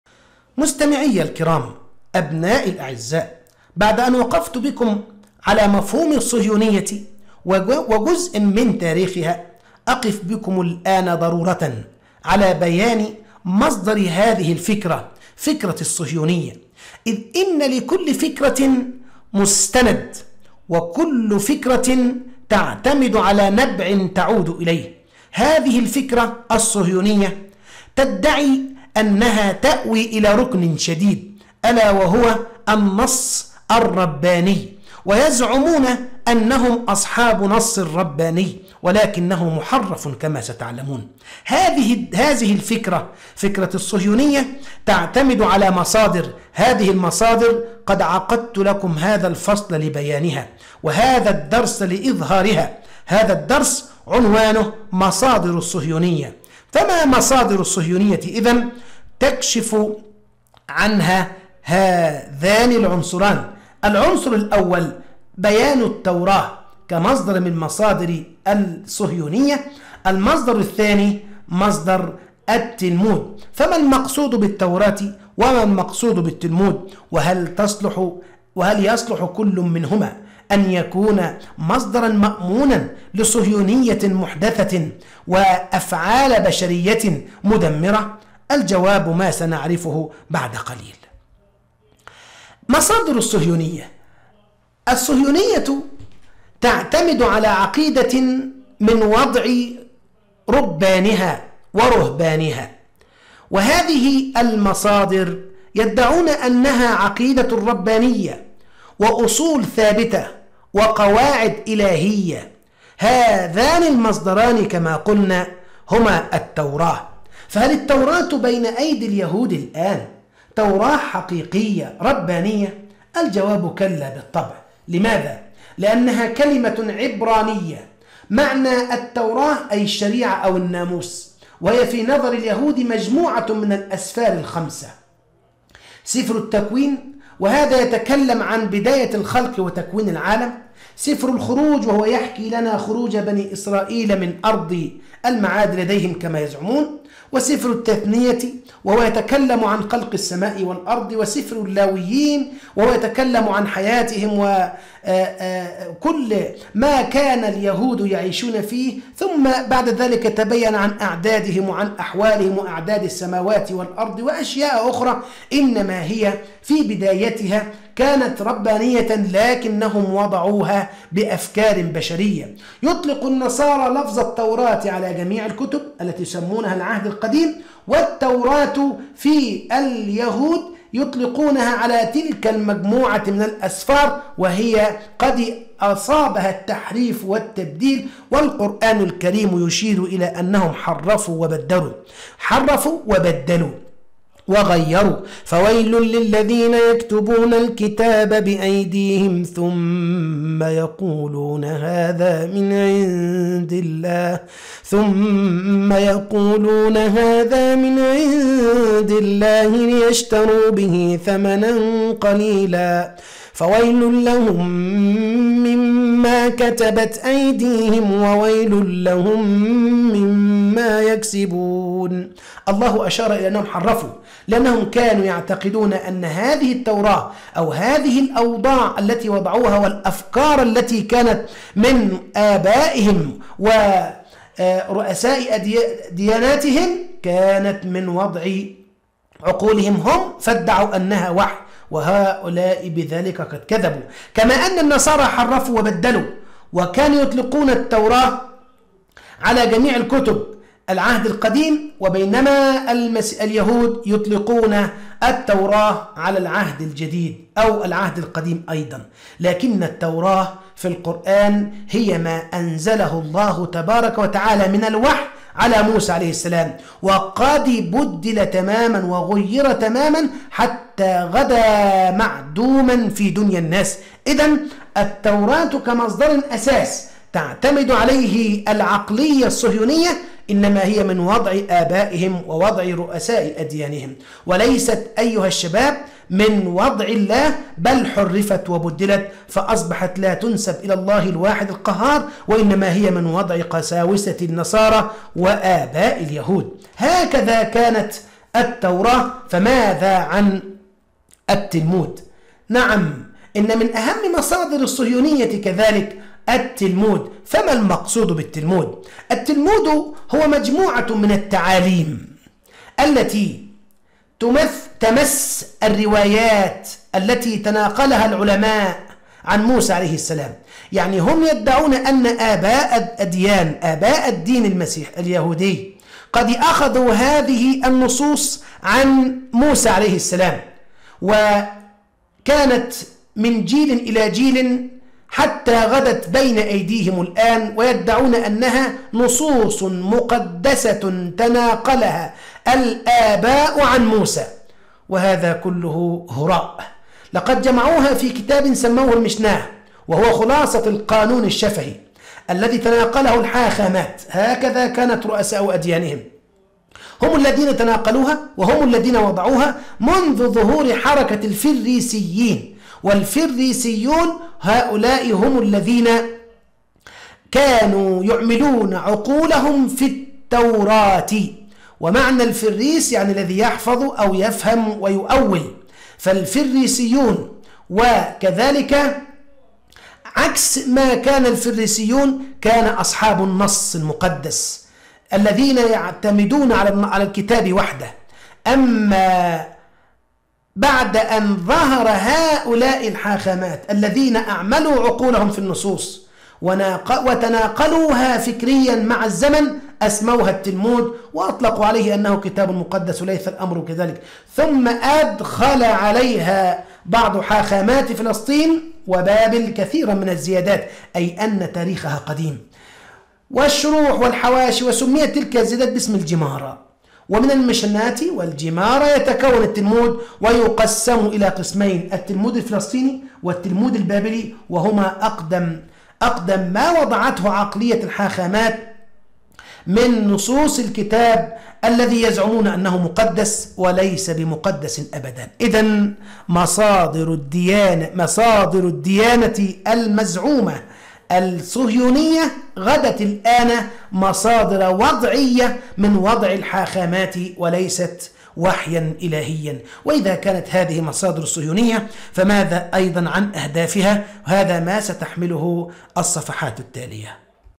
المذاهب الفكرية المعاصرة |5-2| مصادر الصهيونية
المقررات المفتوحة - المذاهب الفكرية المعاصرة
الوحدة 5 : الصهيونية مفهومها - نشأتها – مصادرها –وسائلها –خطرها على العالم الإسلامي
الدرس 2 : مصادر الصهيونية
1) التوراة .
2) التلمود .